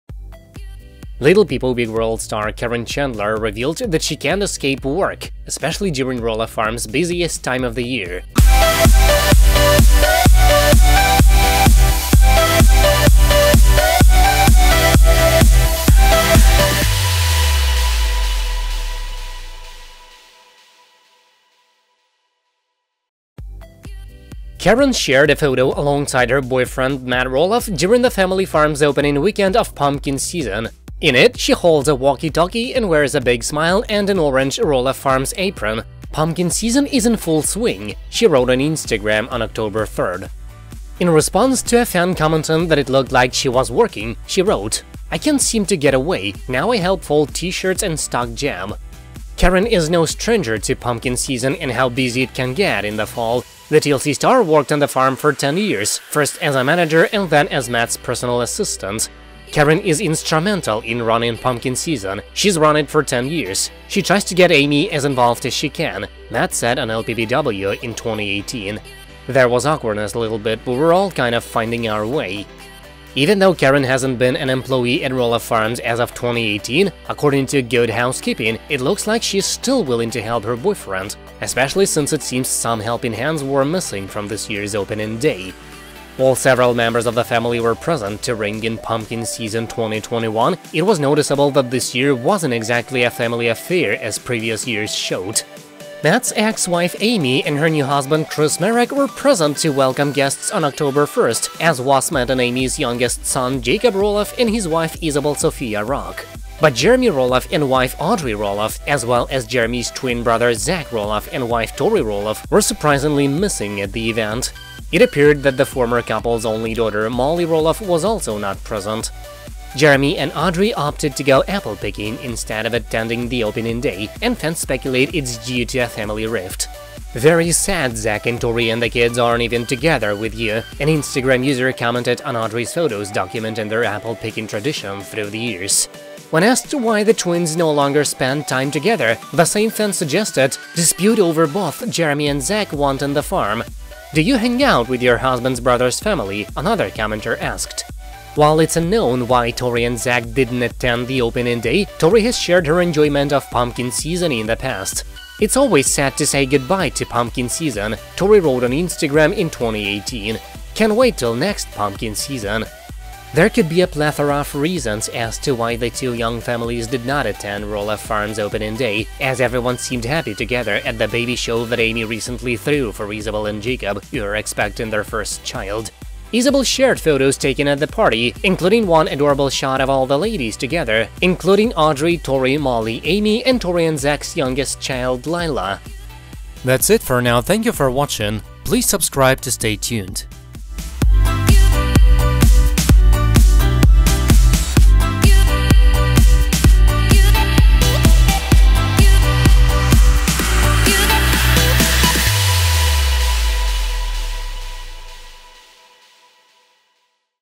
Caryn Chandler Reveals She Still Works at Roloff Farms Amid Family Drama : ‘Can’t Get Away’
Relatable. Little People, Big World star Caryn Chandler revealed that she can’t escape work — especially during Roloff Farms’ busiest time of the year. Caryn, 53, shared a photo alongside her boyfriend, Matt Roloff, during the family farm’s opening weekend of pumpkin season. In it, she holds a walkie-talkie and wears a big smile and an orange “Roloff Farms” apron. “Pumpkin season is in full swing,” she wrote on Instagram on October 3. In response to a fan commenting that it looked like she was working, she wrote, “[LOL.] I can’t seem to get away. Now I help fold T-shirts & stock jam.” Caryn is no stranger to pumpkin season and how busy it can get in the fall. The TLC star worked on the farm for ten years; first, as a manager and then as Matt’s personal */@#$%&*istant. “Caryn is instrumental in running pumpkin season, she’s run it for 10 years. She tries to get [ex-wife Amy Roloff] as involved as she can,” Matt, 59, said on Little People, Big World in 2018. “There was awkwardness a little bit, but we’re all kind of finding our way.” Even though Caryn hasn’t been an employ"